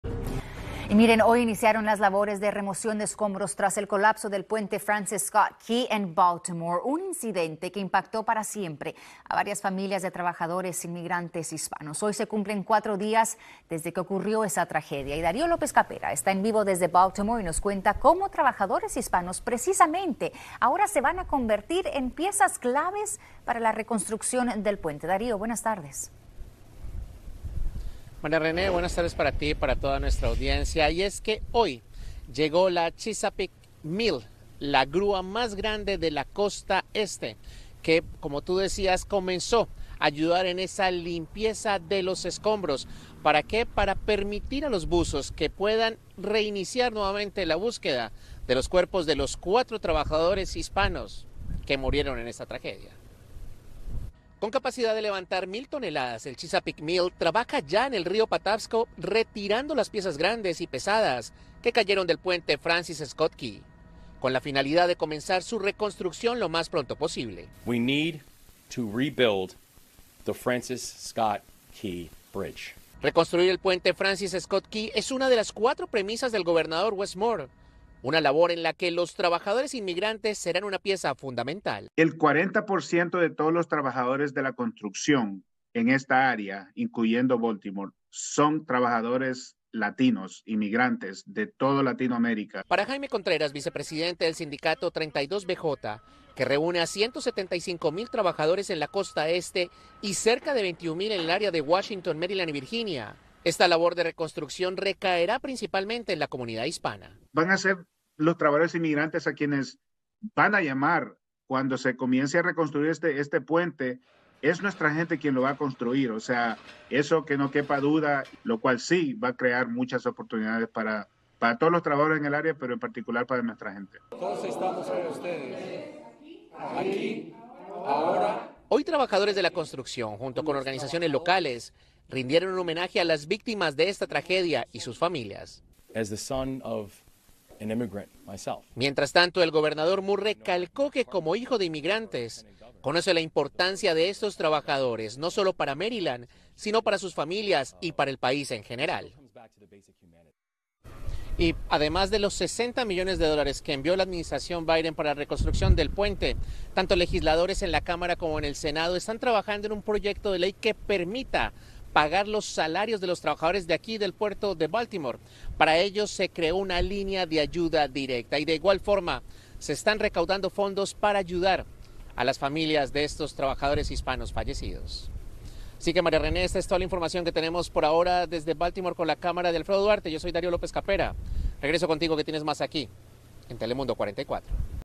Reconstrucción de puente en Baltimore requerirá fuerza laboral hispana | Telemundo 44
La reconstrucción del puente Francis Scott Key en Baltimore podría requerir más fuerza laboral hispana, reconoció el gobernador Wes Moore. Esto es lo que dicen los sindicatos.